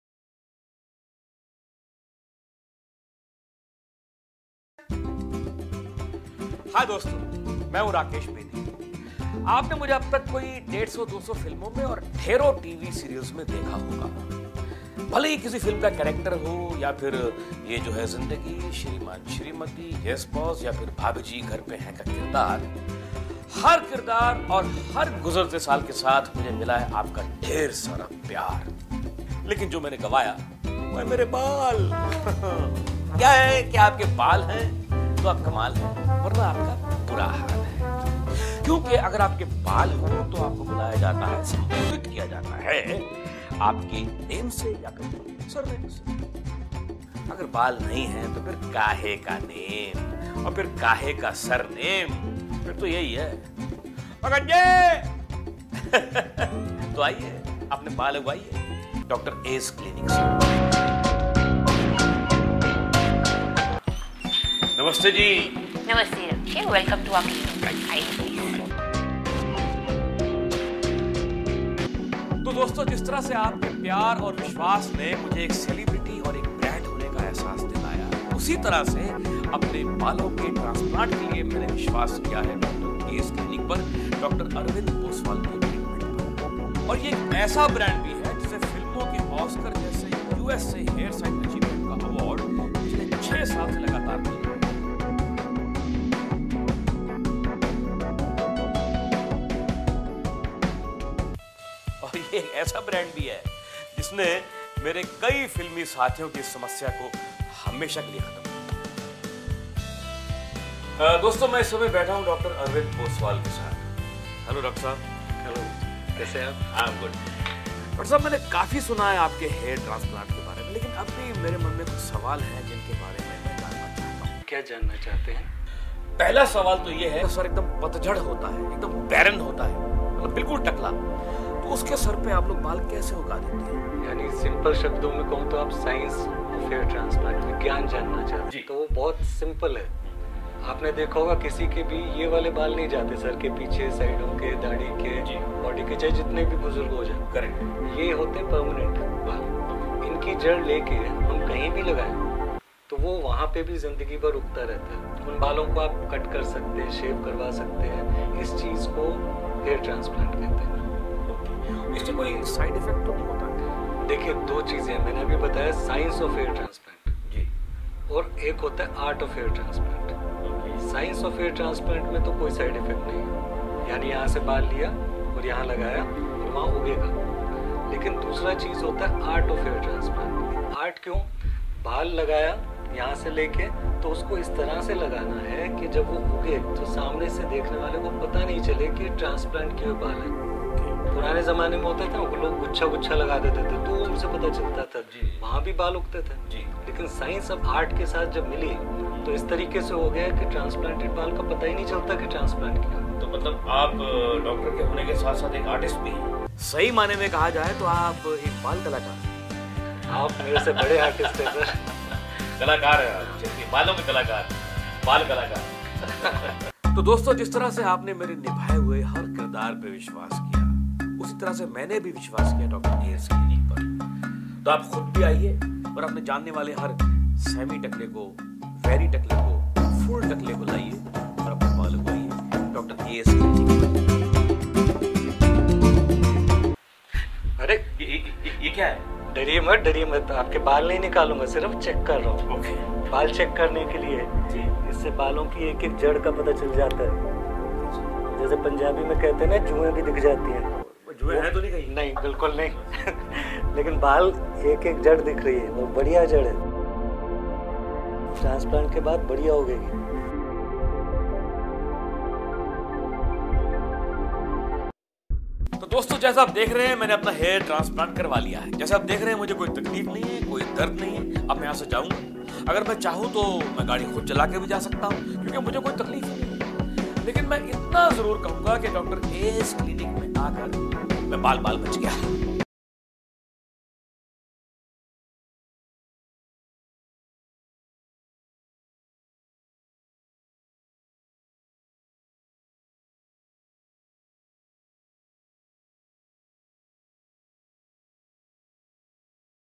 Rakesh Bedi Gets Hair Transplant From Dr. A's Clinic
Famous Actor Rakesh Bedi Gets Hair Transplant From Dr. A's Clinic